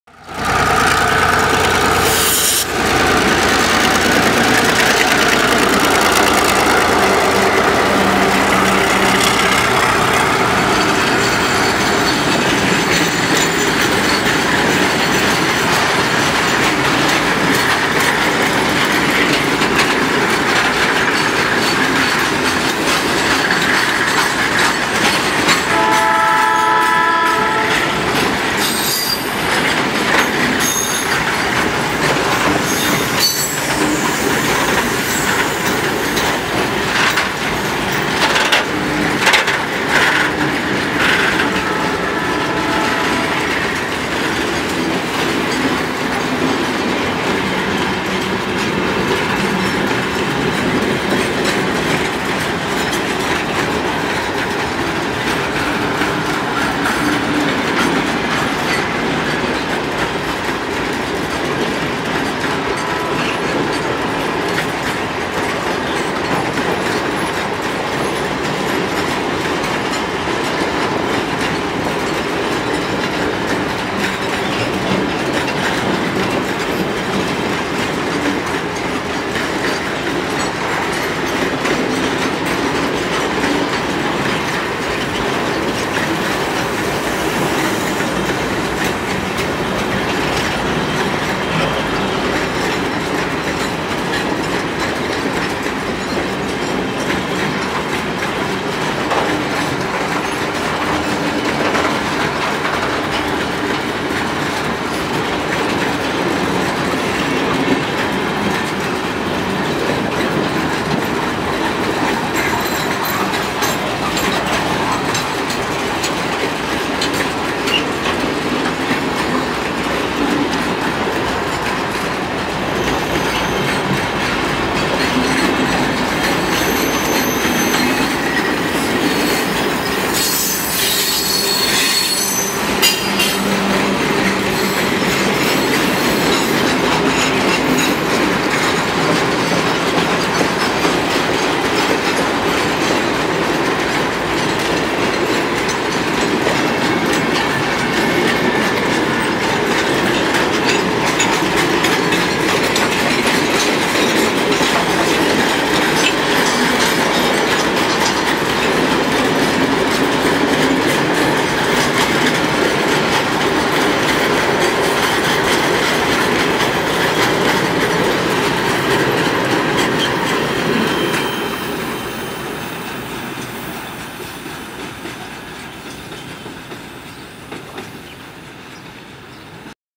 Tren cargado de NCA rumbo a Rosario
En las últimas horas del dia la EMD GR-12W 6627 y la ALCo RSD16 8281 mas un largo tren cargado compuesto por 60 tolvas graneras y 20 tanques aceiteros procedentes de General Deheza van pasando por la Ciudad de Marcos Juárez con rumbo a Rosario. Marcos Juárez. Córdoba. Argentina. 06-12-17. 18:50 hs.